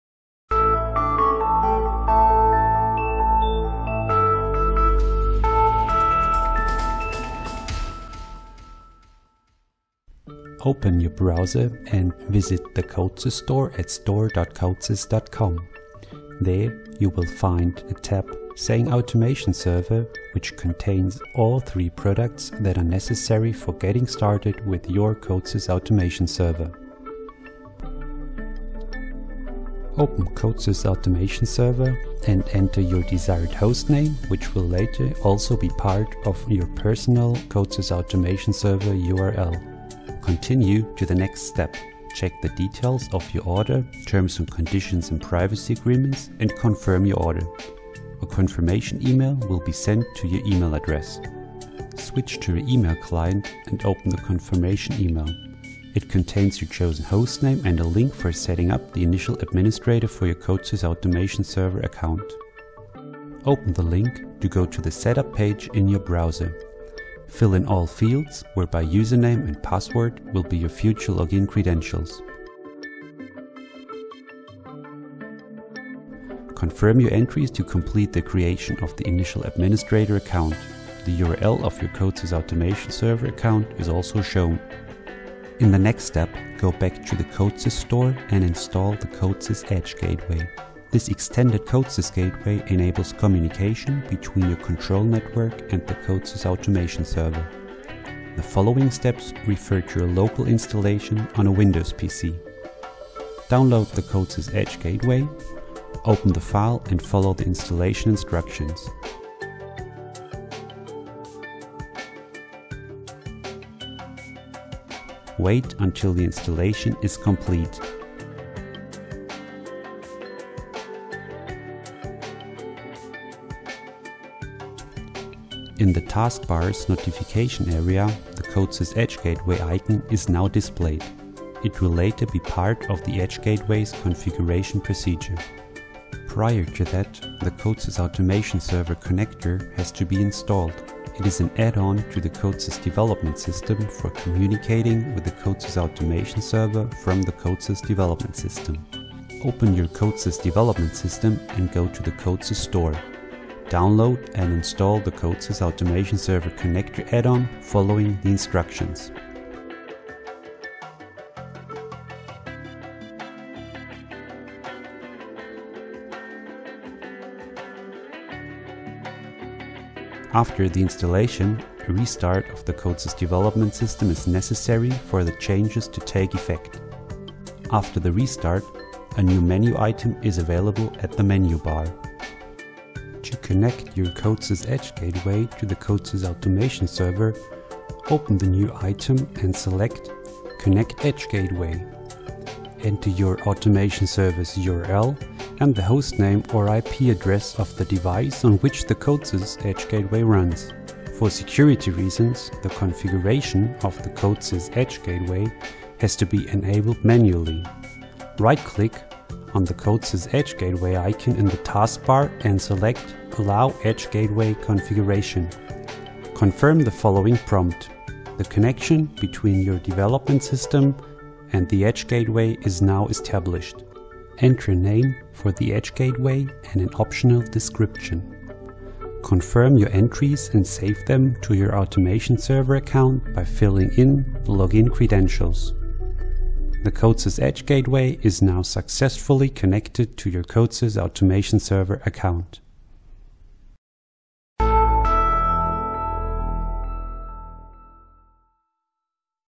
CODESYS Automation Server - Getting started
This video guides you through the first steps that are necessary for getting started with your CODESYS Automation Server.

Content
00:00 - Intro
00:10 - Required products
00:29 - Creating a CODESYS Automation Server account
00:52 - Adding the initial administrator
01:25 - Installing the CODESYS Edge Gateway
02:16 - Installing the CODESYS Automation Server Connector add-on
03:02 - Configuring the CODESYS Edge Gateway

Our customers in the USA, Canada and Mexico, please visit the CODESYS Store North America